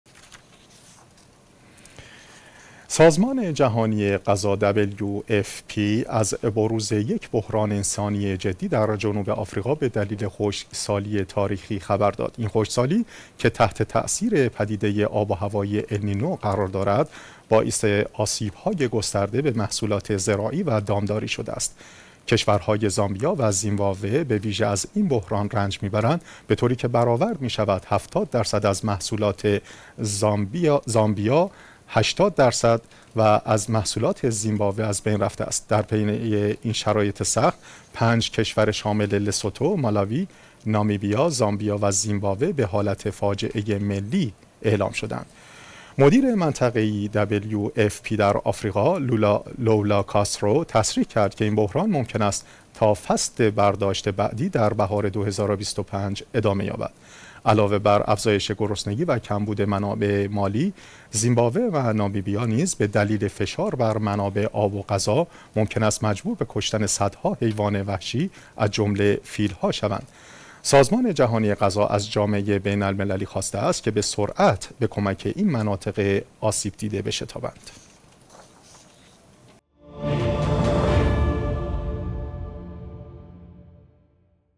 وضعیت فاجعه ملی در زامبی و زیمبابوه: هشدار سازمان جهانی غذا از فاجعه انسانی قریب‌الوقوع!
وضعیت فاجعه ملی در زامبی و زیمبابوه: بحران خشکسالی شدید و طولانی‌مدت در زامبی و زیمبابوه، موجب شده است که این دو کشور رسماً وضعیت فاجعه ملی اعلام کنند.
 خشکسالی بی‌سابقه که تحت تأثیر پدیده النینو قرار گرفته، بیش از 70 درصد محصولات زامبی و 80 درصد محصولات زیمبابوه را نابود کرده است.
 این وضعیت نه تنها بحران غذایی را تشدید کرده، بلکه فشار بی‌سابقه‌ای بر منابع طبیعی از جمله آب و دامداری وارد کرده است. جوامع محلی با خطر گرسنگی گسترده و تخریب معیشت خود مواجه هستند.

اما آیا وضعیت فاجعه ملی در زامبی و زیمبابوه تنها آغاز یک بحران انسانی بزرگتر در آفریقاست؟ آیا منابع بین‌المللی برای نجات این کشورها کافی خواهد بود؟
برای بررسی بیشتر این وضعیت بحرانی و تأثیرات گسترده آن بر محیط زیست و انسان‌ها، شما را دعوت می‌کنیم که گزارش کامل ویدئویی را مشاهده کنید. پاسخ این سوالات و حقایقی که شاید شما را شوکه کند، در این ویدئو بررسی می‌شود!

ویدیوهای مرتبط

ویدیوهای مرتبط